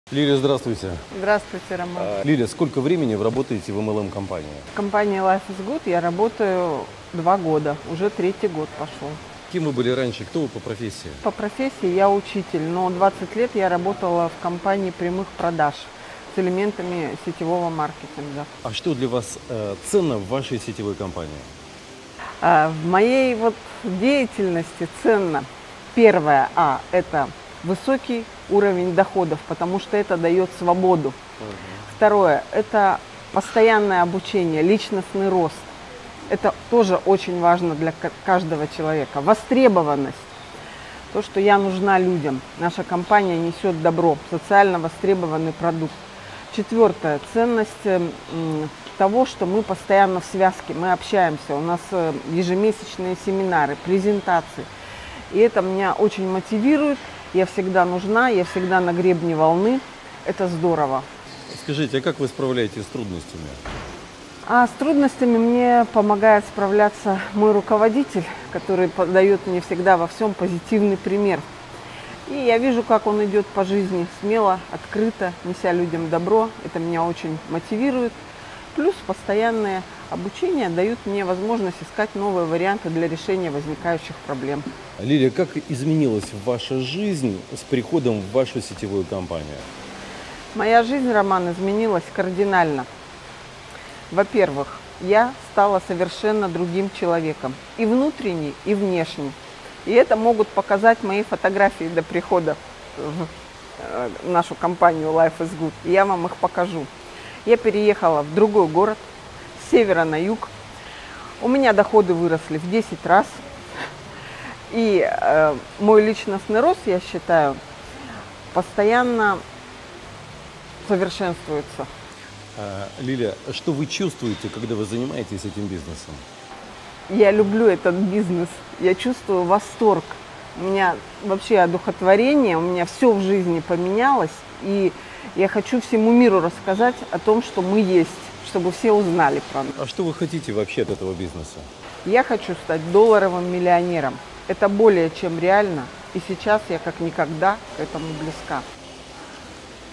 ЧТО ДАЁТ СЕТЕВОЙ БИЗНЕС какие выгоды получаешь, работая в многоуровневом маркетинге
Друзья, сегодня интервью с настоящей трудягой и очень позитивным, добрым человеком, Лилией Щербаковой. Лилия начинала в компании LifeisGood с нуля, за 2 года многого достигла, и сейчас в одном шаге от директора. В этом интервью Лилия рассказала о себе, о своей жизни до и после начала работы в сетевой компании, о работе в LifeisGood, про то, как справляется с трудностями, и о планах на будущее.